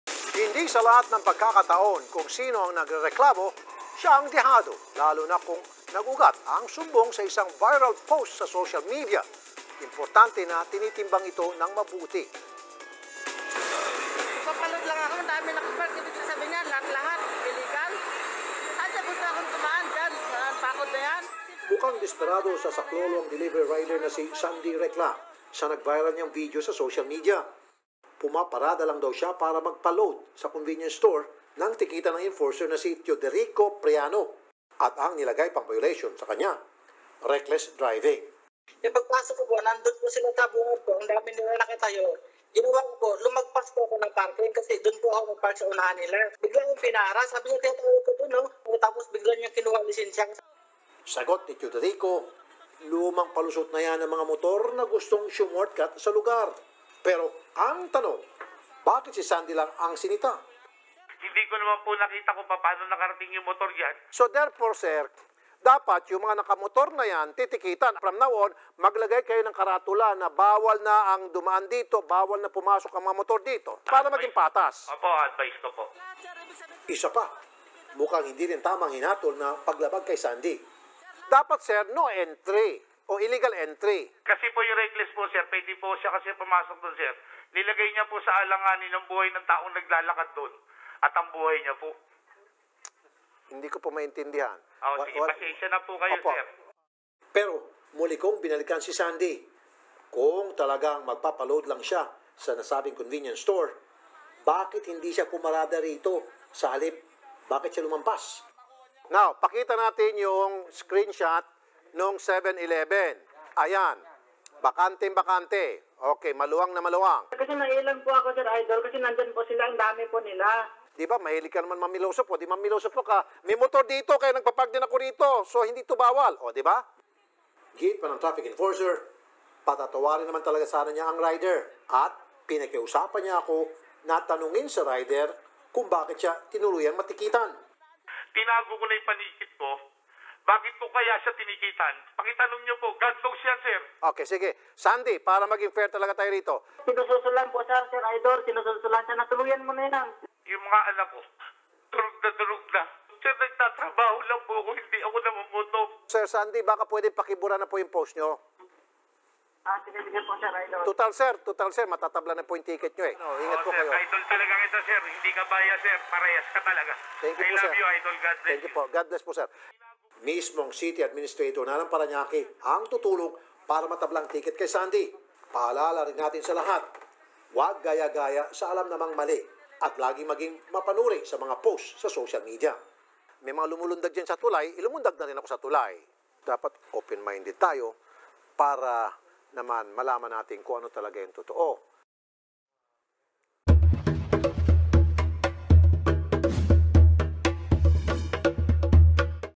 Rider, inireklamo ang paniniket ng traffic enforcer
ActionMan | Viral sa social media ang reklamo ng isang rider tungkol sa paniniket sa kanya ng isang traffic enforcer dahil sa iligal na pagparada. May mali nga ba ang enforcer?